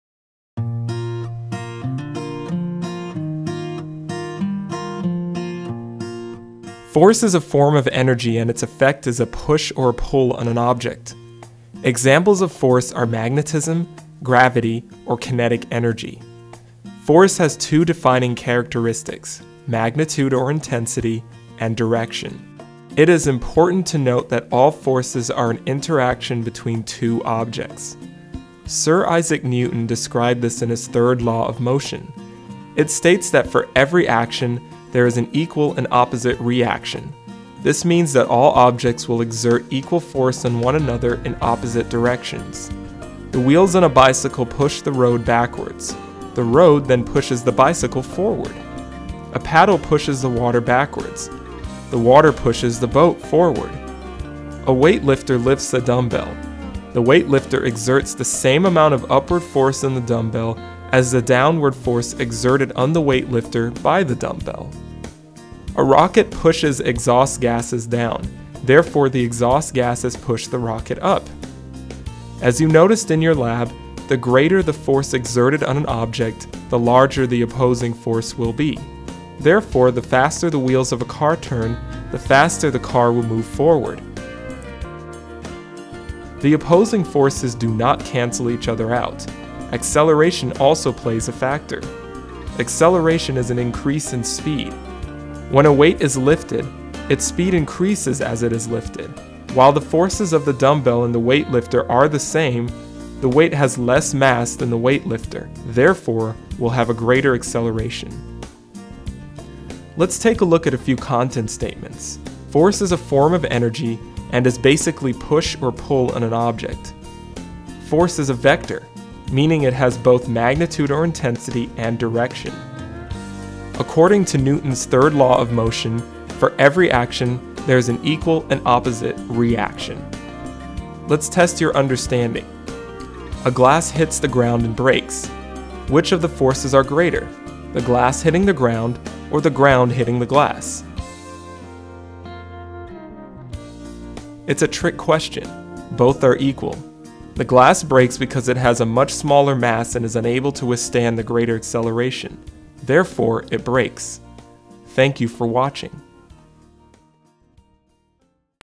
SC.3.P.10.2 Action and Reaction
Explains the science behind the Laws of Motion and forces.

Florida Sunshine State Standards SC.3.P.10.2:
Recognize that energy has the ability to cause motion or create change.